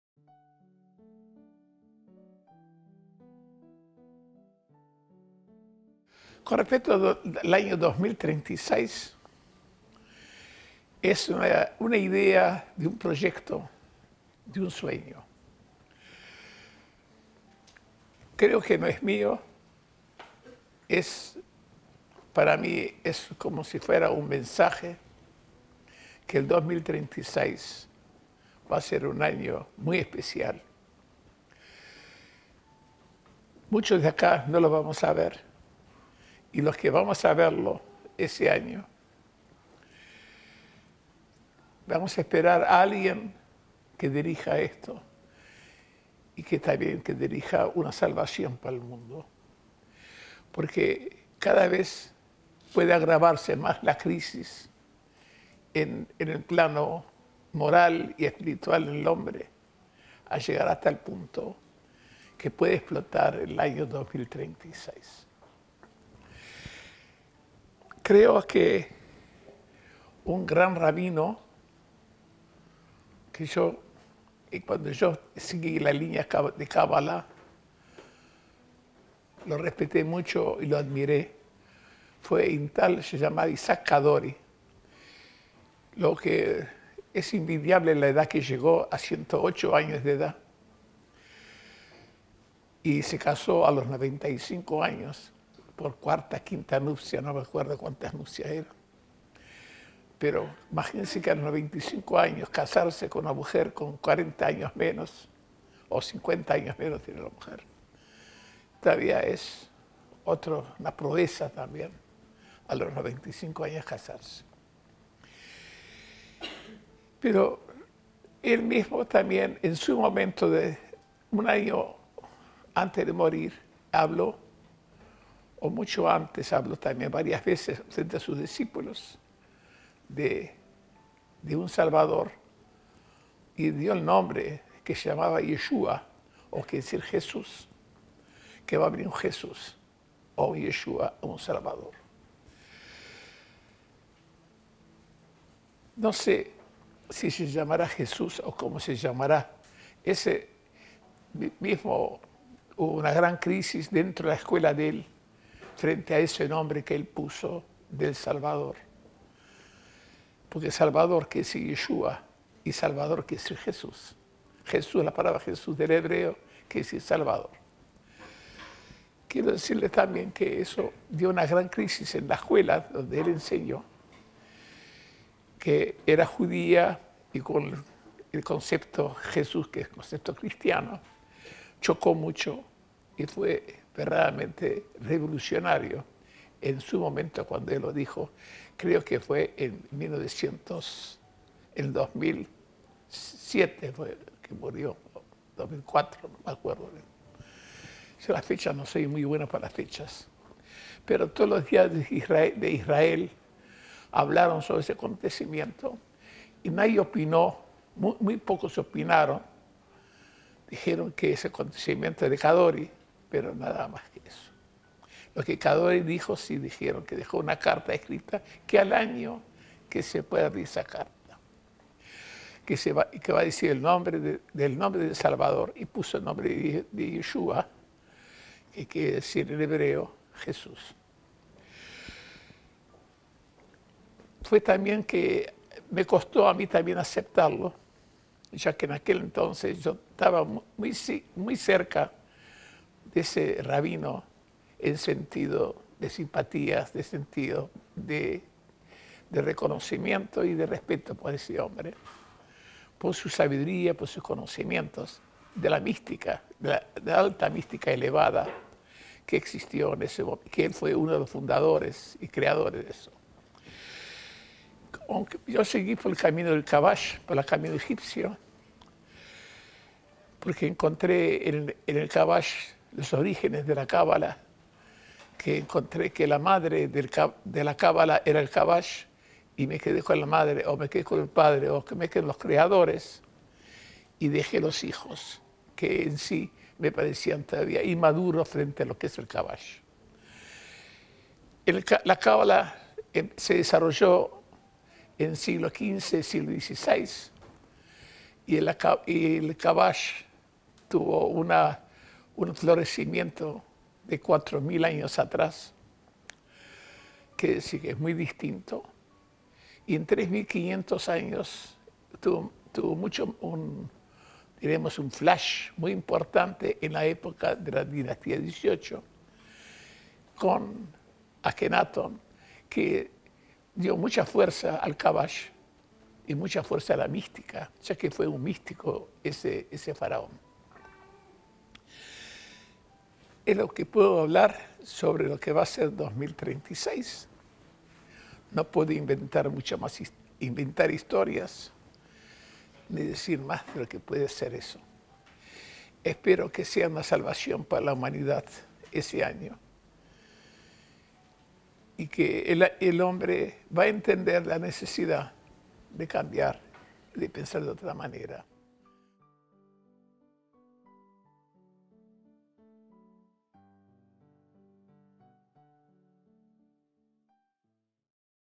AÑO 2036
Es un sueño de un años mesiánico. Es un mensaje que he recibido en que en esa fecha se dará un gran cambio para el hombre.Puede llegarse a una gran crisis mundial que el hombre deberá tomar consciencia de que necesita cambiar su forma de vivir.
Lo anunció también Kadori un gran rabino a quien el Maestro Rolland,admiró quien habló de que iba a volver Jesús, como un nuevo salvador para el mundo.